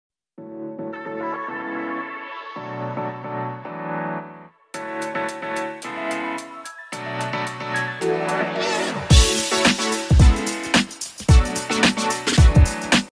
The Greater Victoria Harbour Authority wishes you a Happy New Year and all the best in 2024!
GVHA stewards the Breakwater in Victoria BC, Canada. In this video our staff walk the Breakwater, enjoying the sights above and below the water.